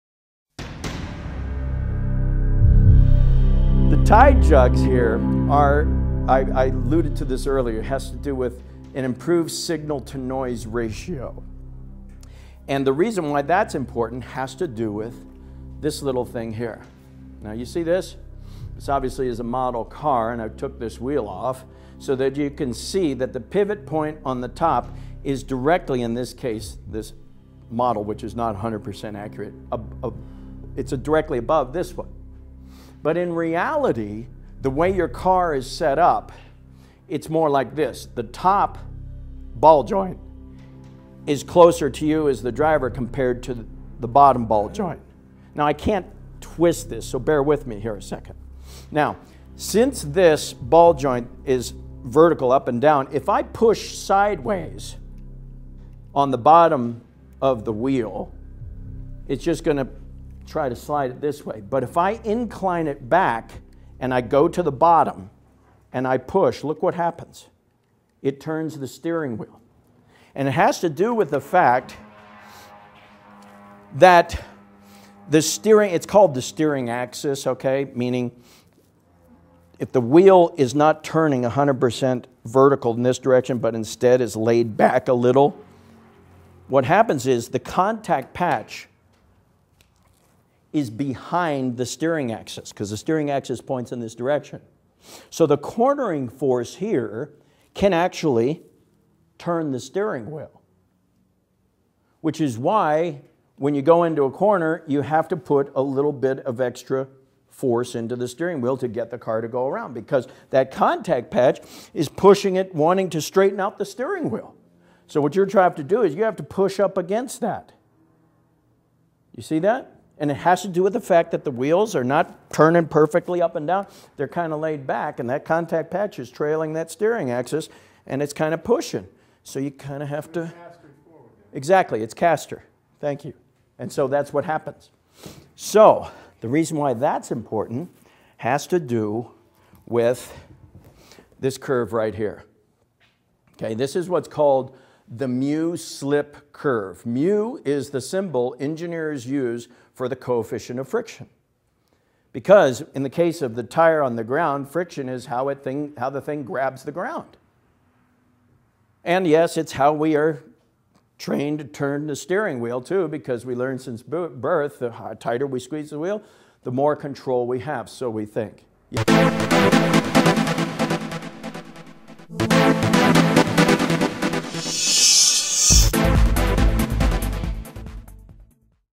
Steering Wheel Feedback At the Cornering Limit
| Steering wheel feedback in a race car boils down to cornering force due to steering caster which puts the contact patch behind the steering axis.  So, resistance is your friend when you're out on the track.  Your goal is to find the steering wheel angle with the most resistance and stay there for the shortest period of time while seeking the shortest distance to the finish line.